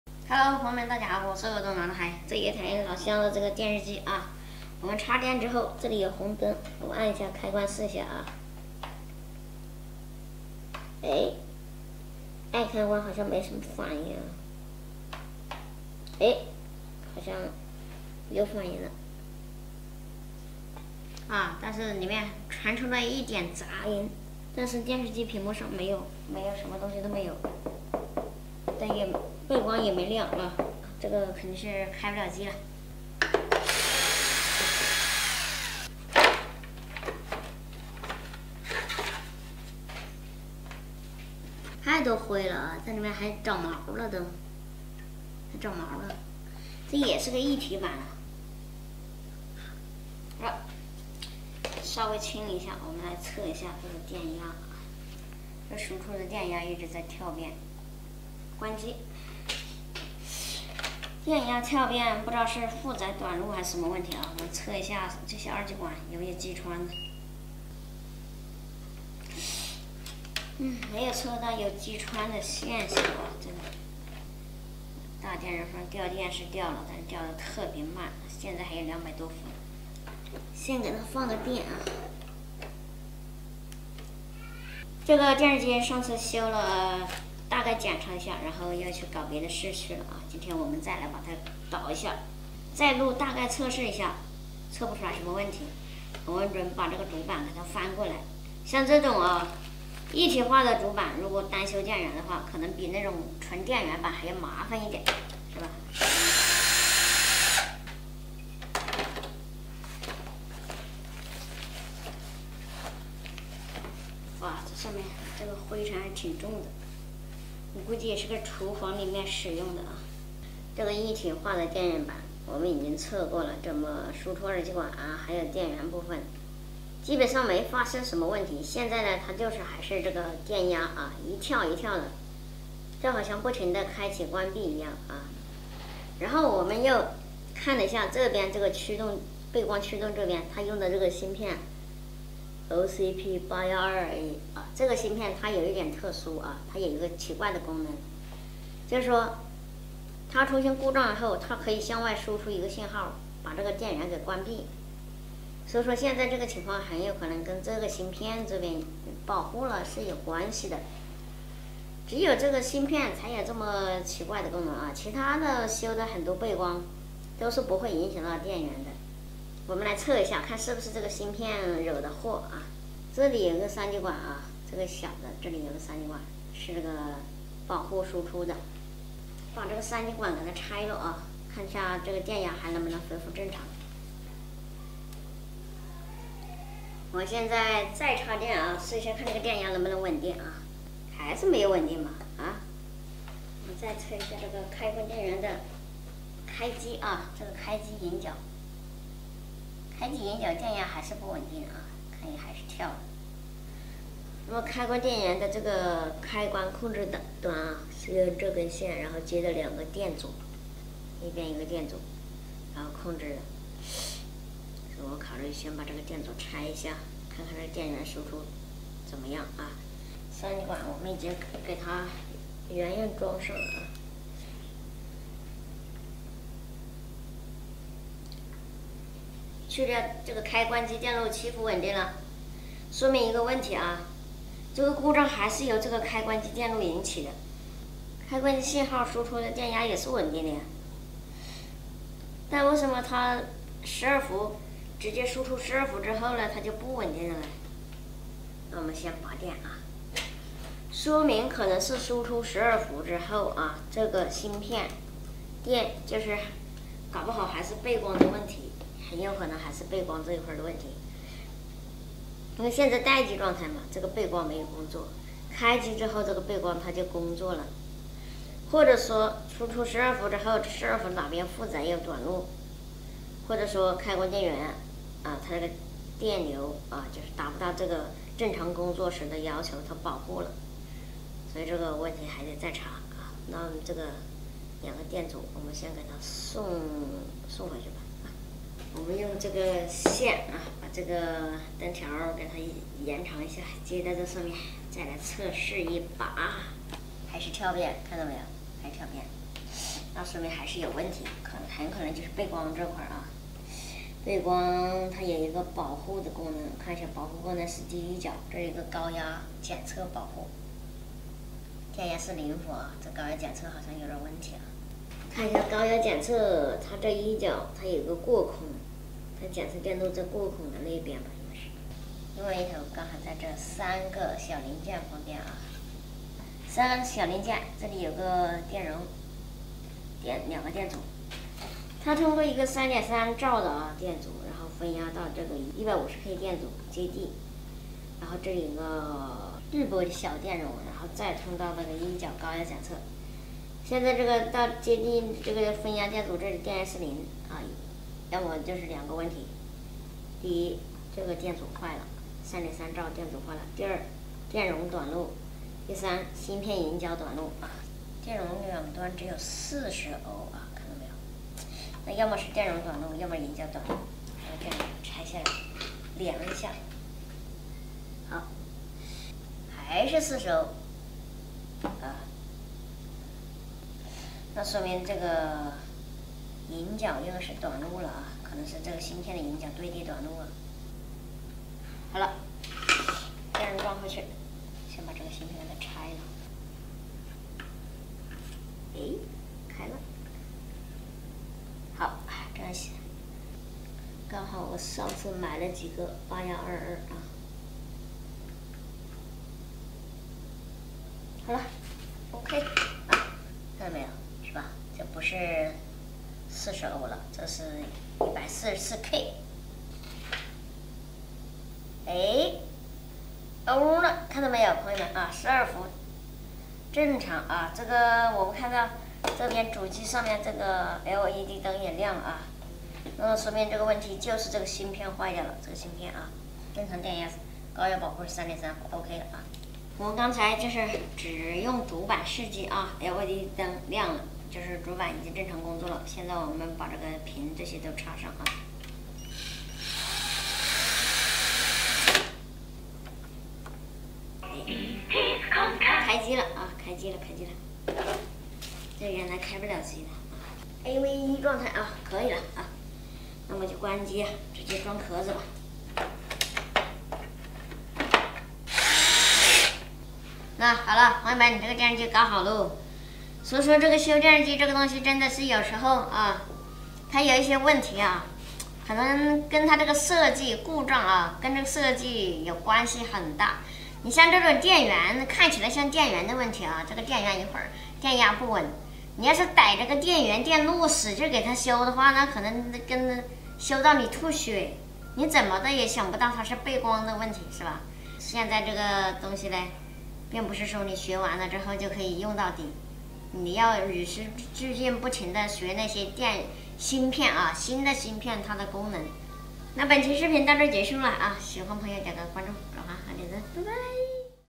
这台液晶电视不开机，老男孩一番检修只换了一个芯片，满血复活了
鄂东老男孩【鄂东老男孩】vlog，老乡送来一台康佳液晶电视不开机，老男孩费老劲查出问题，原来只换一颗芯片就修复了，看似几分钟，实际是知识的不断积累，正应了那句学无止境！